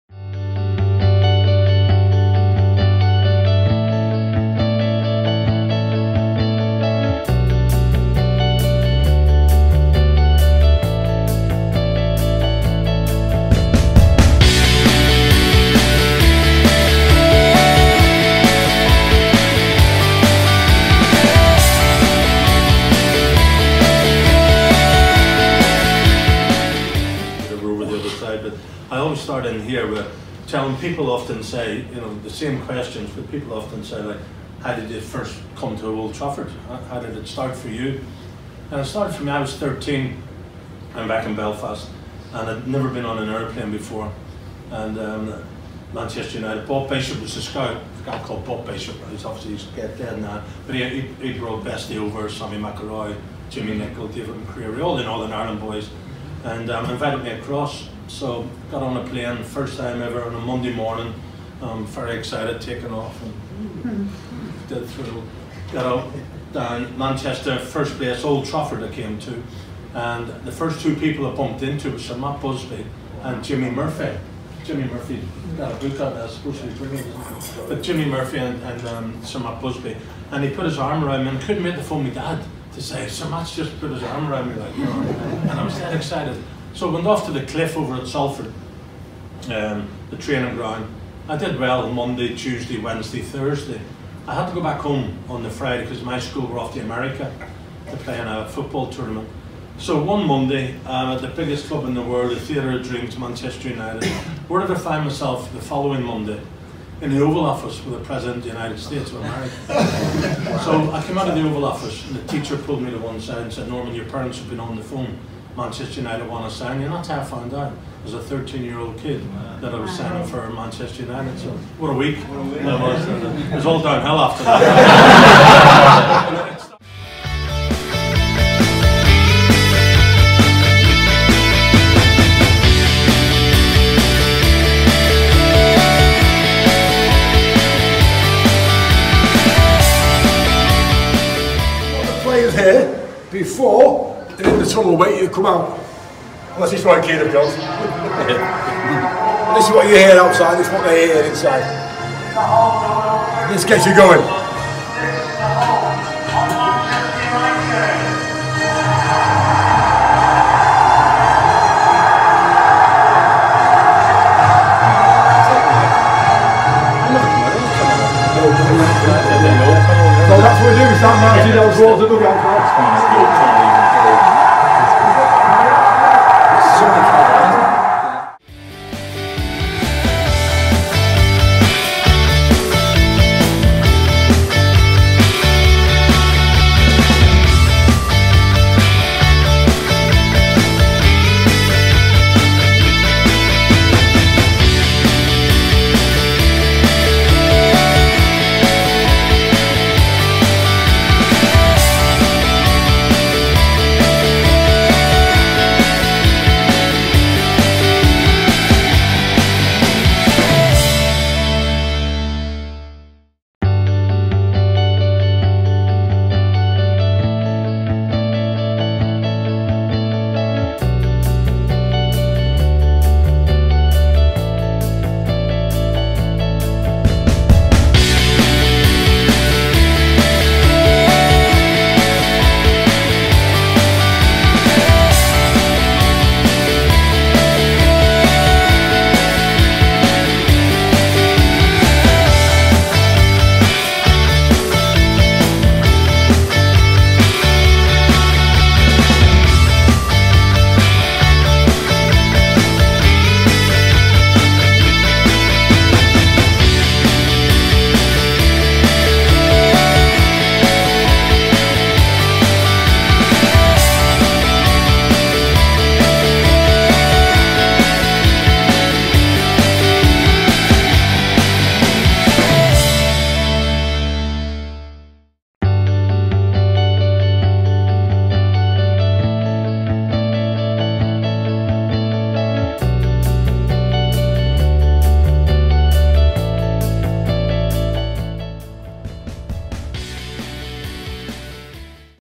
MEETING NORMAN WHITESIDE! I Manchester United: Legends Tour
Happy Sunday!
Today I Took My Boyfriend To Manchester For The Manchester United LEGENDS Tour! ⚽️

Hope You Guys Like It And Comment What Other Videos You Would Like To See Below! :)